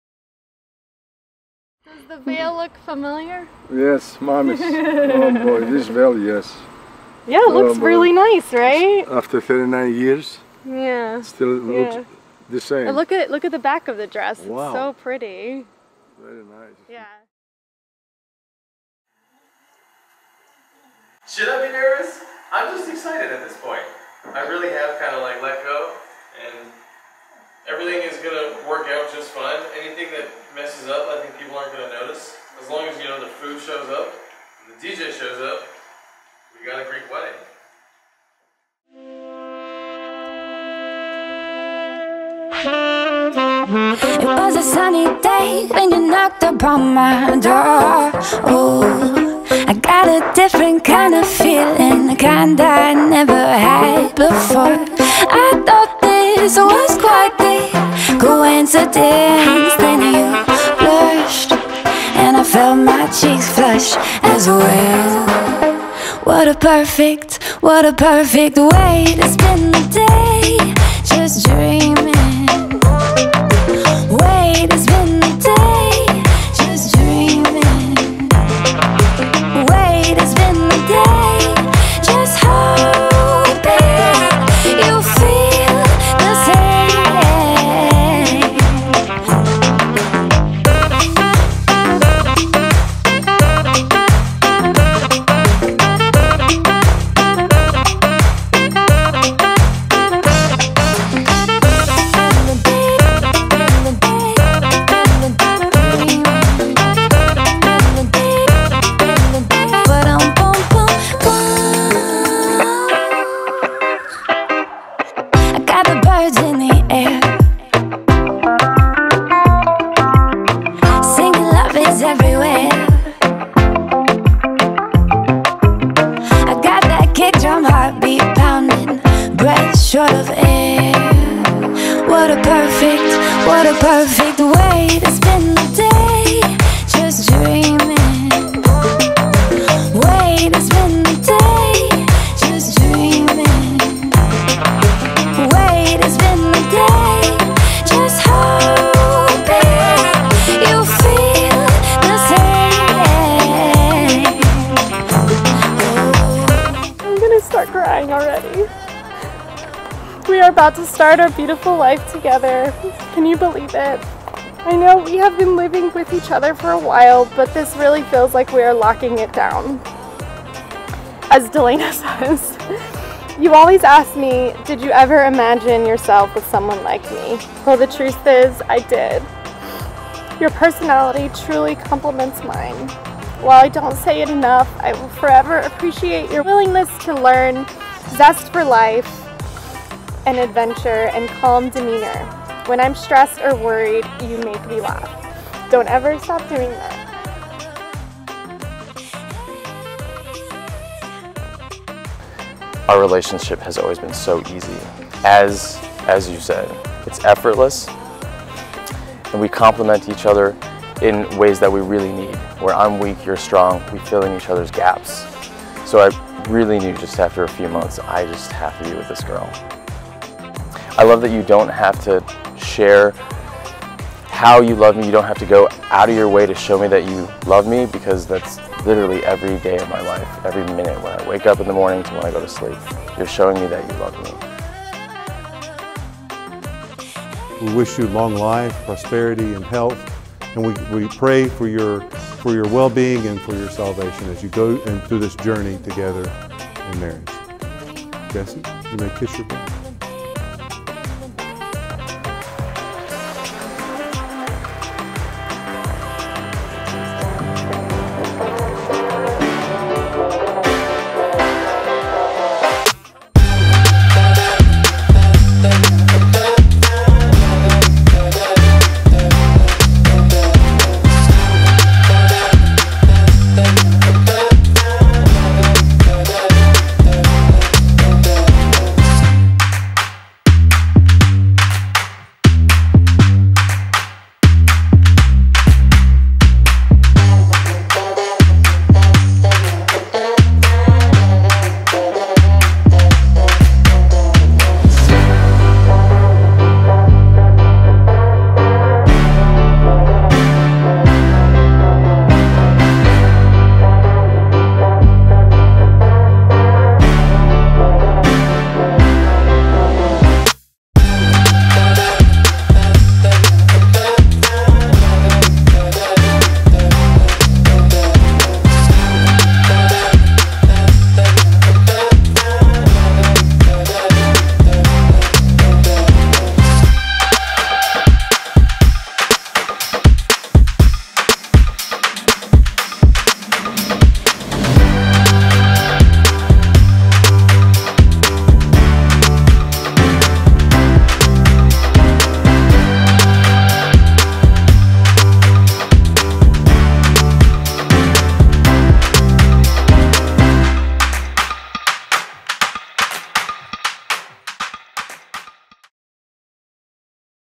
Greek Wedding | North Carolina | Elina + Jesse
Elina and Jesse were married in the Greek church that Elina has been a member of for nearly 30 years. It was a hot September day in Winston Salem, NC, but that didn't stop the celebration of their love with all of their family and friends. Enjoy all of the tears and laughter from this gorgeous wedding day!